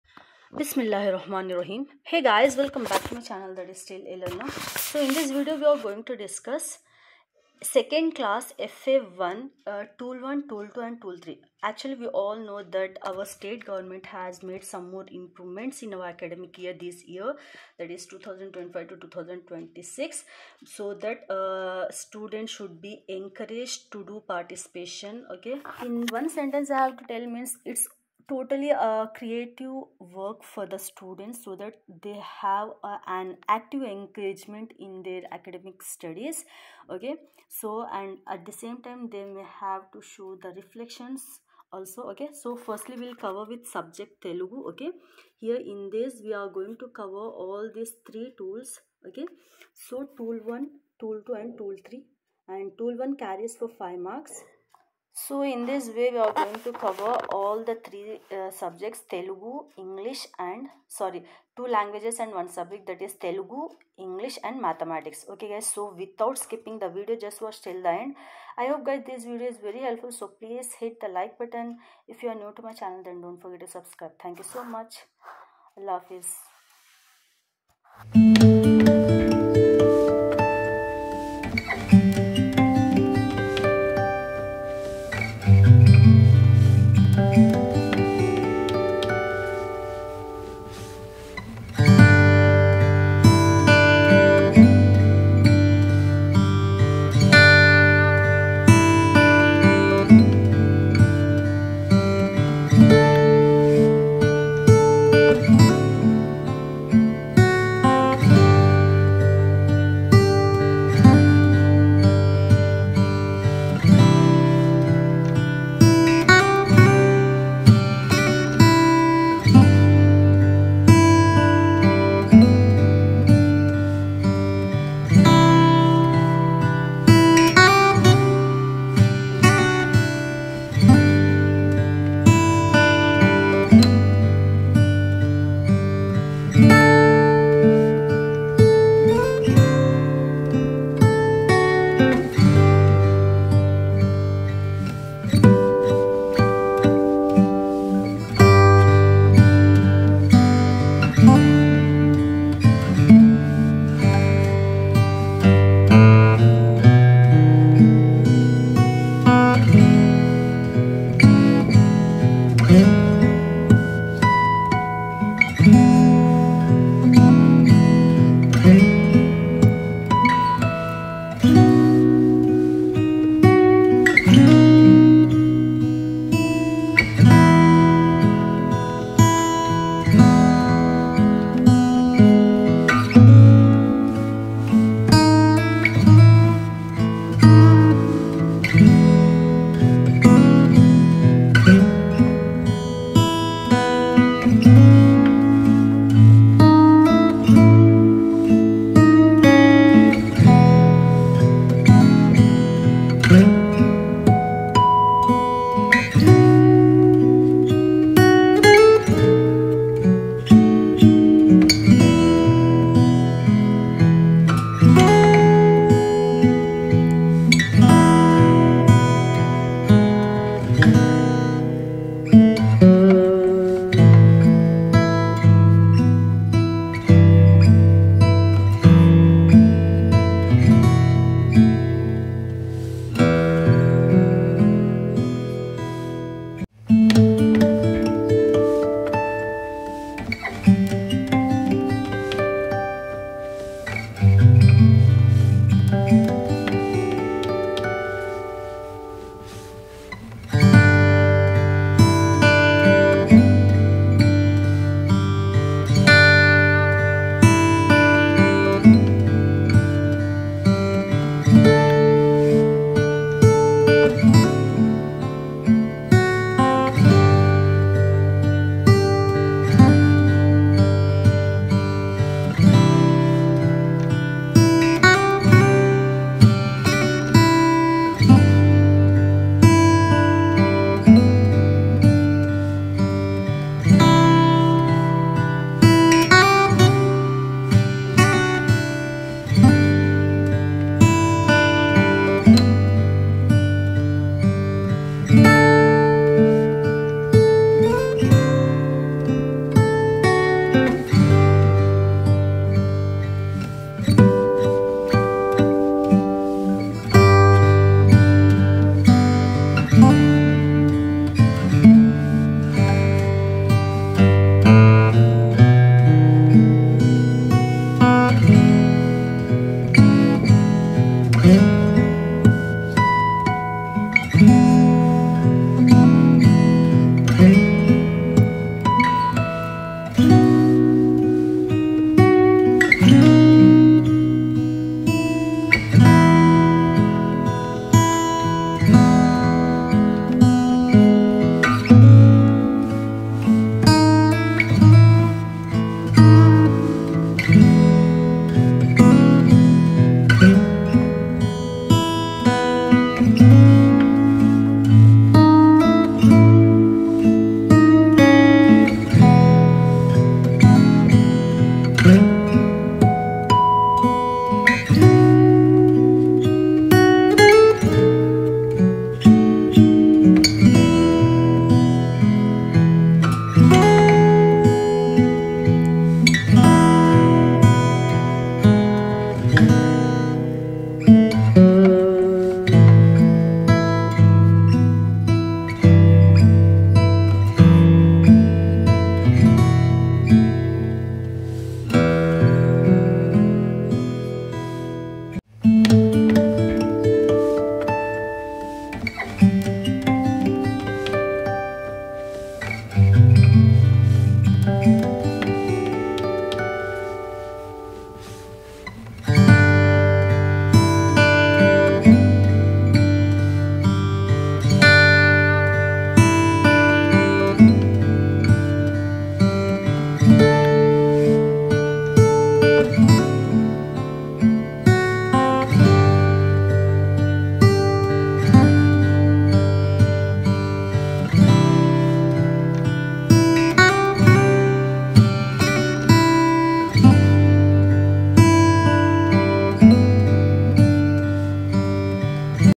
2nd Class -"Tool 1, Tool 2, Tool 3 - MATHS, Telugu, English" | 2nd Class FA-1 Tools | AP Schools👍🗞️
"Tool 1, Tool 2, Tool 3 -2nd  Class-MATHS, Telugu, English" | FA's Tool 1-Model Student Participation and Reflection | FA Maths Tool 1 - Student Participation and Reflections

Checkout my Whatsapp for new updates:-